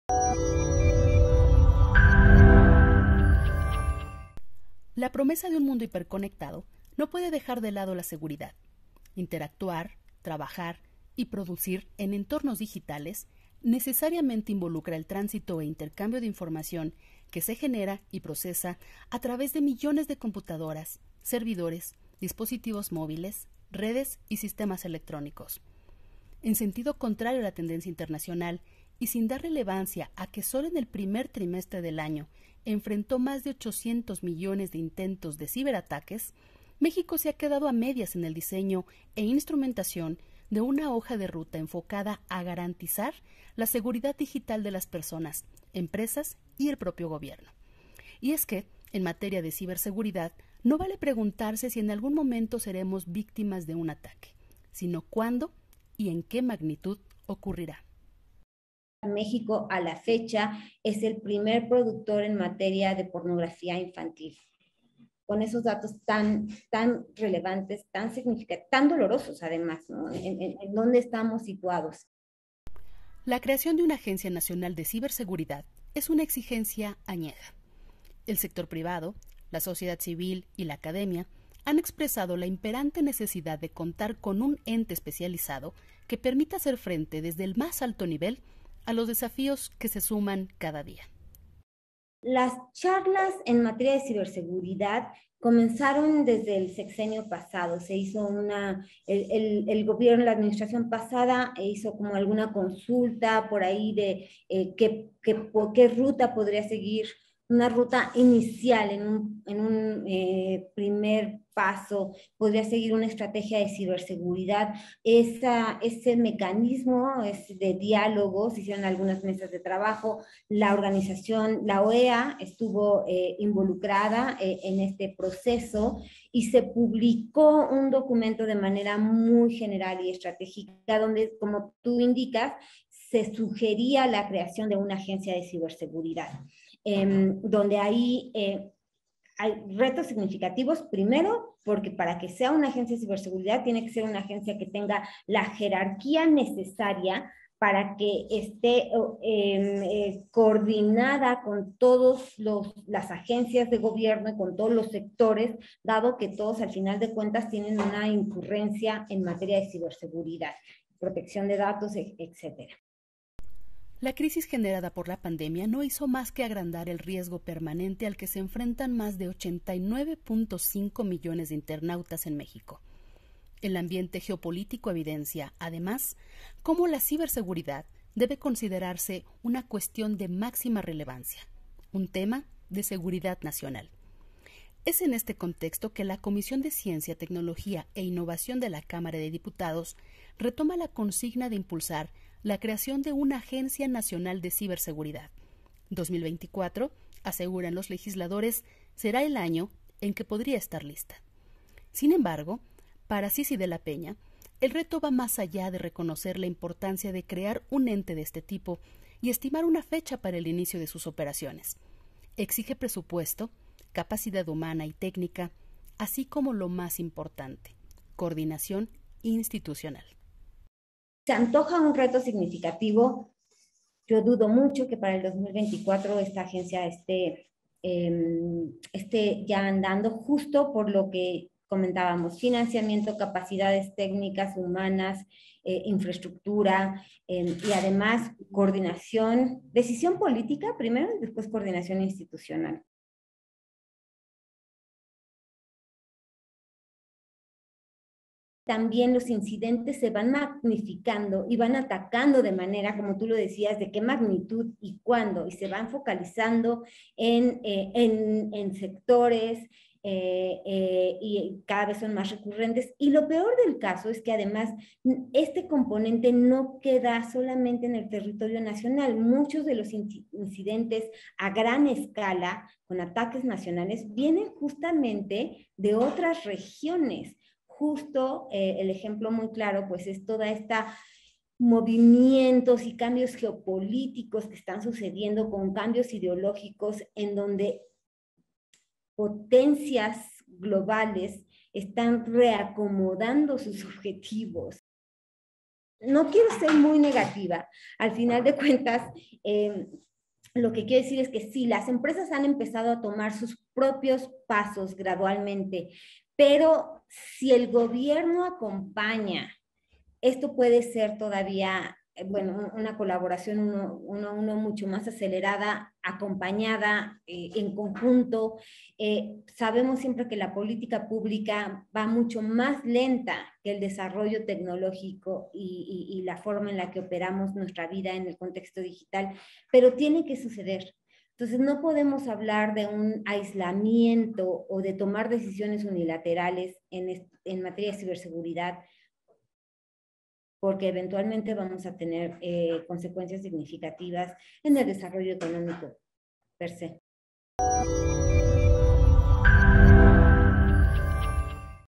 México y su estrategia de ciberseguridad, ¿lenta y nada segura?
La promesa de un mundo hiperconectado no puede excluir la seguridad. Interactuar, trabajar y producir en entornos digitales, necesariamente involucra el tránsito e intercambio de información que se genera y procesa a través de millones de computadoras, servidores, dispositivos móviles, redes y sistemas electrónicos.

En sentido contrario a la tendencia internacional y sin dar relevancia a que sólo en el primer trimestre del año enfrentó más de 800 millones de intentos de ciberataques, México se ha quedado a medias en el diseño e instrumentación de una hoja de ruta enfocada a garantizar la seguridad digital de las personas, empresas y el propio gobierno.

Y es que, en materia de ciberseguridad no vale preguntarse si en algún momento seremos víctimas de un ataque, sino cuándo y en qué magnitud ocurrirá.

De esto conversamos con Sissi de la Peña, Directora para México y centroamérica y gerente regional de Comercio Digital de la Asociación Latinoamericana de Internet (ALAI).